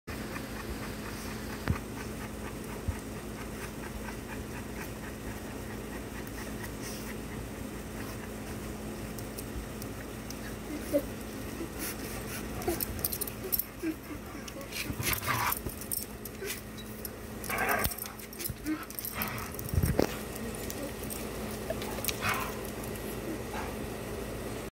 Pit Bull loves kisses from Einstein
Einstein loves giving Tank kisses